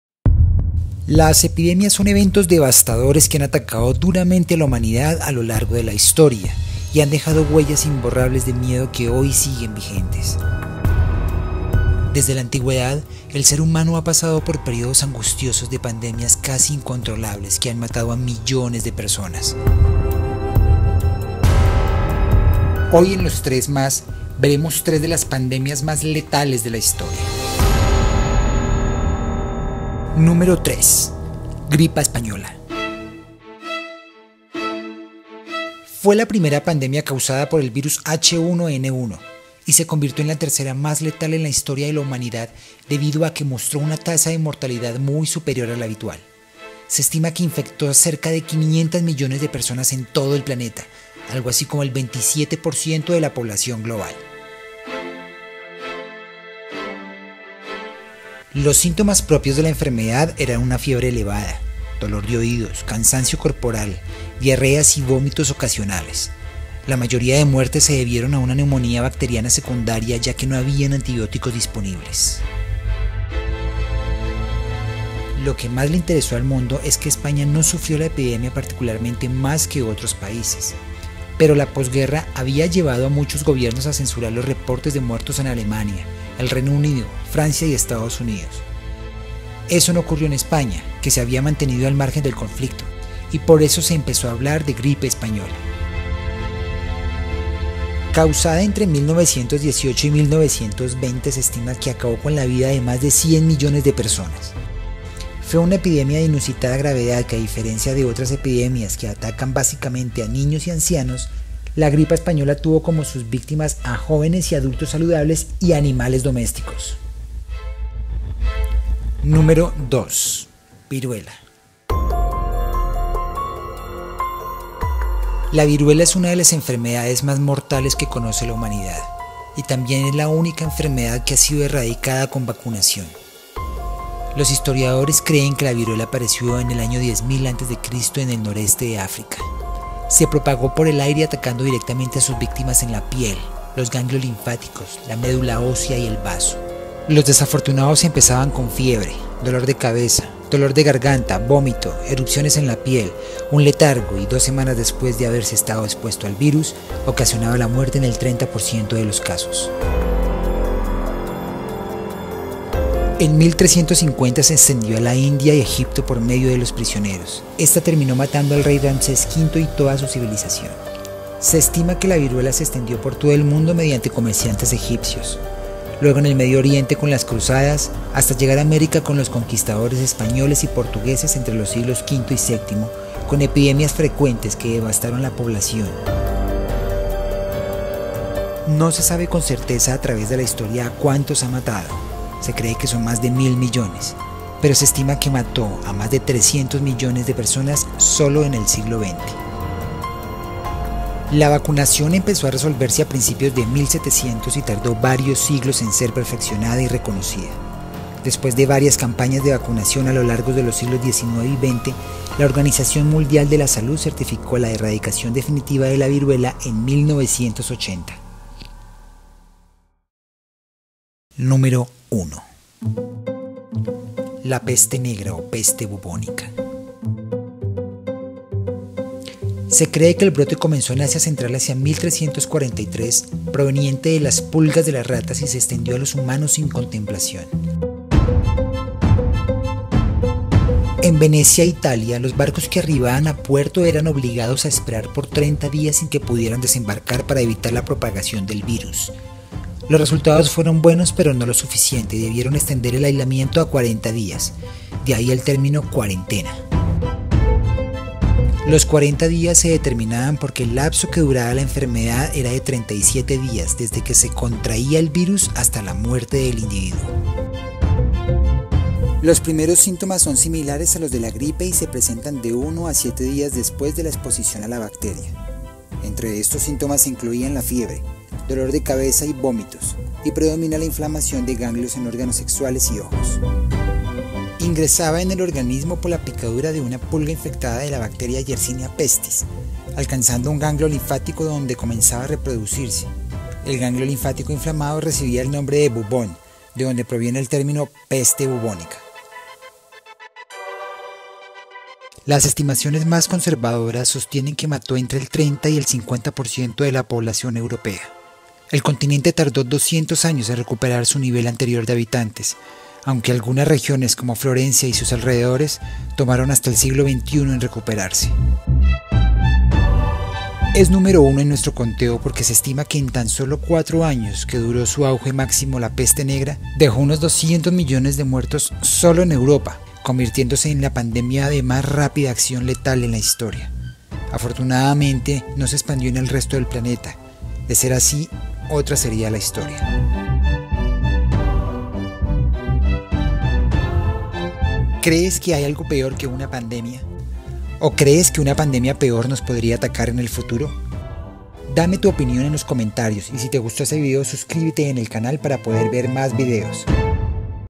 ☠️ LAS EPIDEMIAS MÁS LETALES DE LA HISTORIA ☠️
Las tres epidemias mas letales por las que ha pasado la humanidad, si hemos sobrevivido a estas fatalidades podremos sobrevivir al COVID-19.